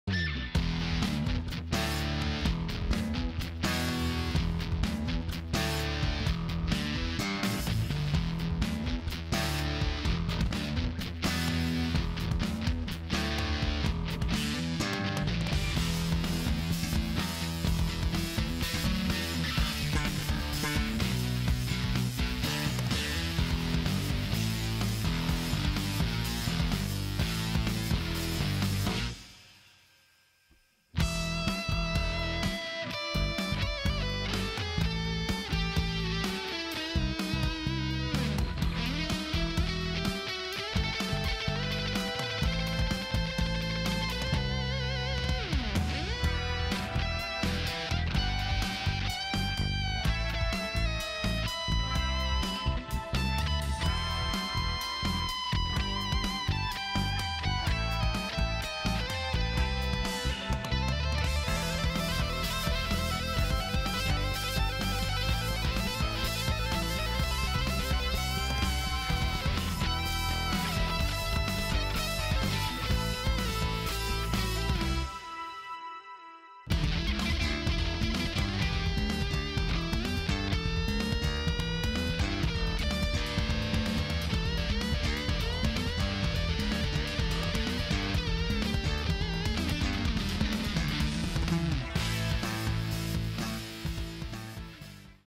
Intervento di Inserimento impianti con varie tecniche e macchinari
Intervento di mini rialzo crestale, utilizzando il Magnetic Mallet e la chirurgia Piezo Eletrica, Questo permetterà di effettuare un intervento minimamente invasivo, rispettando tessuti molli e duri.
Intervento realizzato dal Dottor Roberto Provasoli dello studio Bellora Provasoli Via Luigi Borghi, 14, 21013 Gallarate VA.